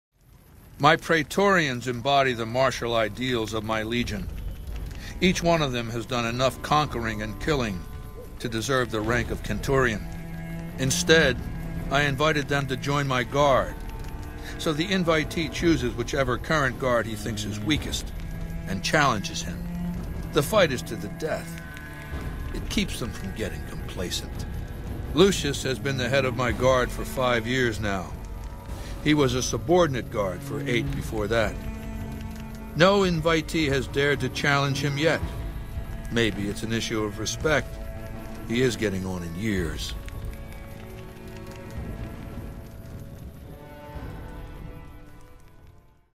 Caesar's Praetorian Guard
Caesar explains certain intricacies of his personal guard. How they're chosen, and how they earn a place in the elite guard unit.